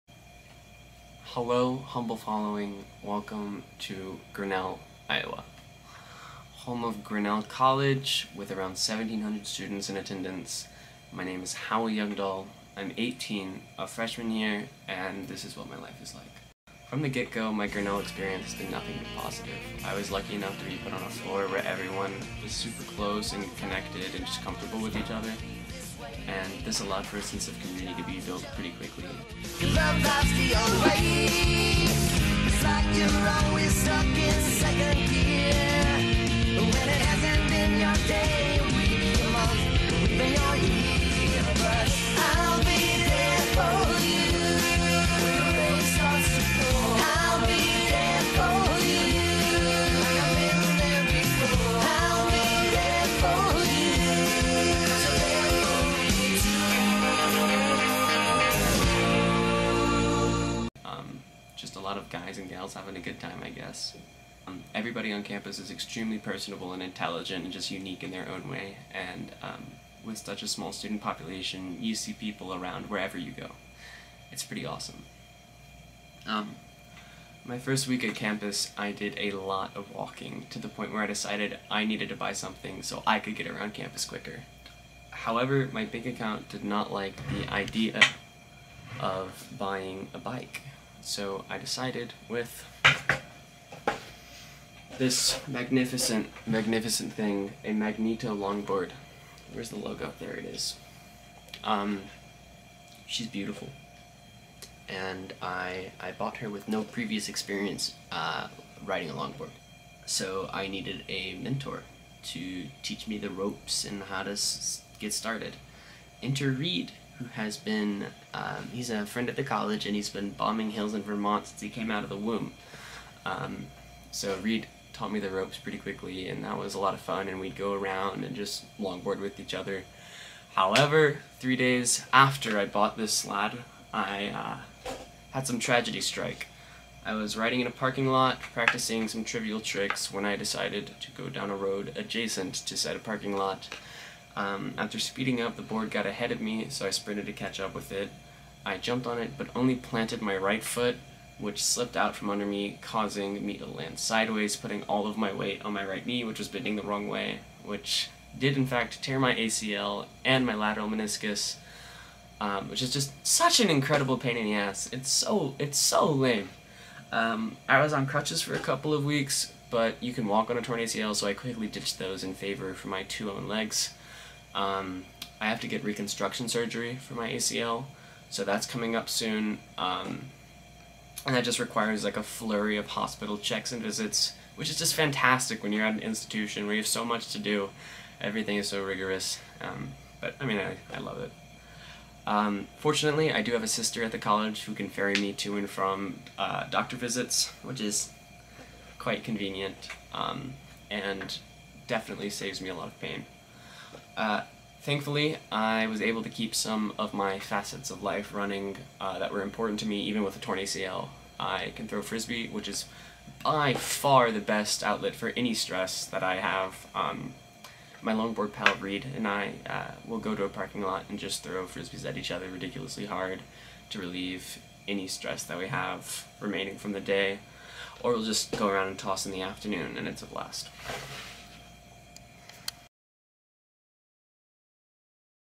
How people are reacting to Grinnell
Sup gang - welcome to Grinnell College. In this video, I go over what I have been up to since I've moved to Grinnell, Iowa to go to school. Unfortunately a couple weeks after I got here, I tore my ACL longboarding, which prohibits me from playing on the Ultimate Frisbee team and longboarding, coincidentally the two activities I want to do most. I introduce my lads and show some footage around the college, talking about my life and experiences thus far.

college, college humor, college vlog, grinnell, grinnell college, grinnell iowa,